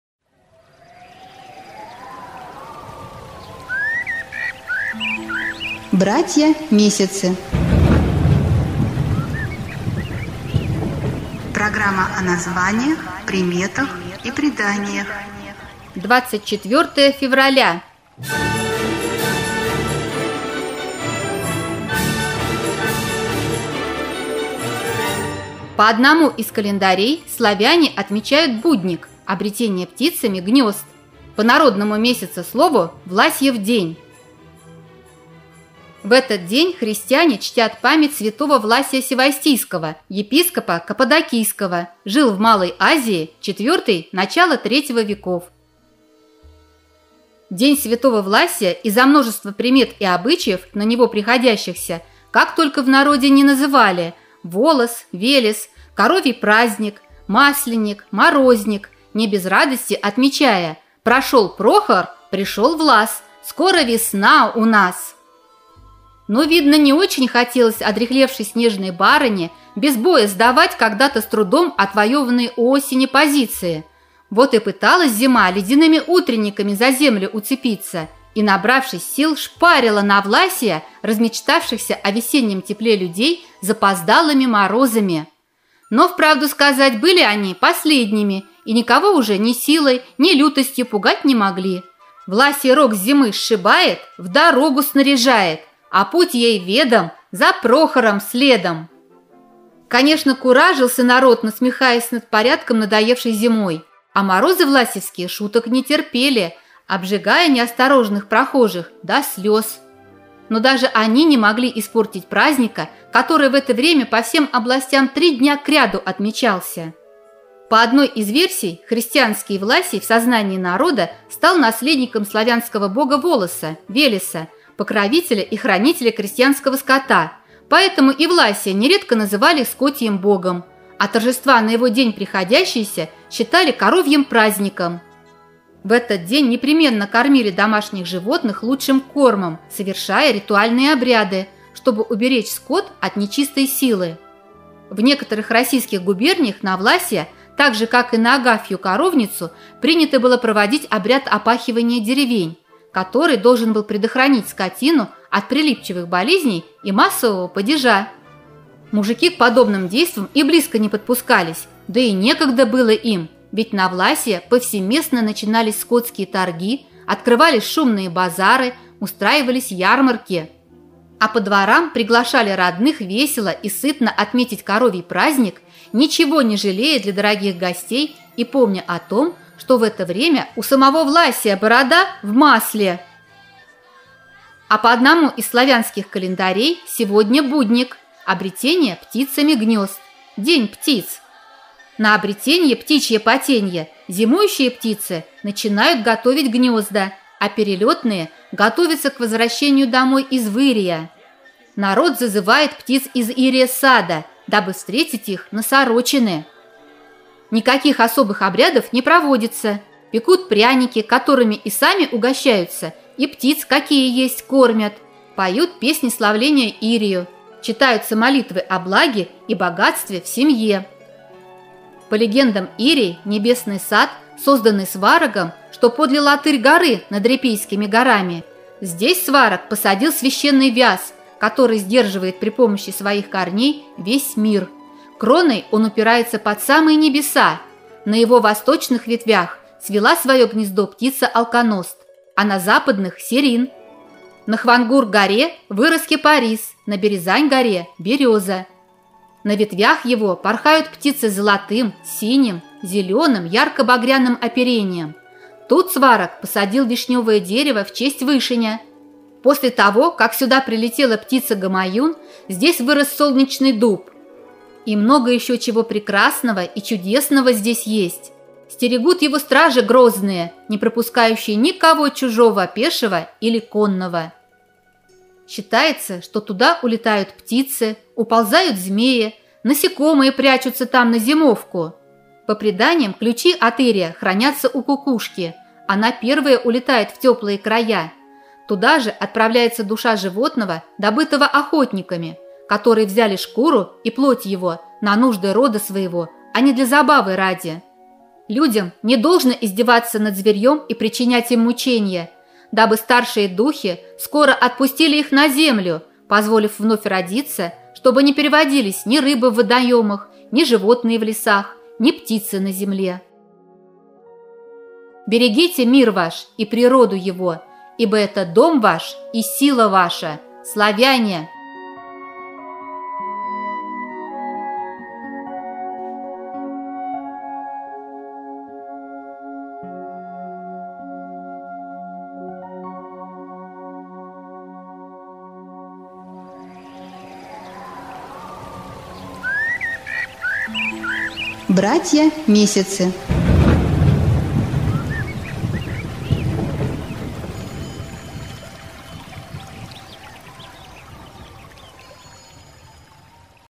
Братья-месяцы. 24 февраля - Власьев день
По одному из календарей славяне отмечают Будник - обретение гнёзд. По народному месяцеслову: Власьев день.

День святого Власия из-за множества примет и обычаев, на него приходящихся, как только в народе не называли: Волос, Велес, Коровий праздник, Масленик, Морозник, не без радости отмечая: «Прошел Прохор, пришёл Влас — скоро весна у нас»…

…куражился народ, насмехаясь над порядком надоевшей зимой, а морозы Власьевские шуток не терпели, обжигая неосторожных прохожих до слез. Но даже они не могли испортить праздника, который в это время по всем областям 3 дня кряду отмечался…

…По одной из версий, христианский Власий в сознании народа стал «наследником» славянского Бога Волоса (Велеса), покровителя и хранителя крестьянского скота, поэтому и Власия нередко называли Скотьим Богом…

…В некоторых российских губерниях на Власия, так же как и на Агафью Коровницу, принято было проводить обряд опахивания деревень…

…по одному из славянских календарей сегодня Будник - обретение гнёзд. День птиц…

…Народ зазывает птиц из Ирия сада, дабы встретить их на Сорочины…

Продолжение слушайте в сетке вещания…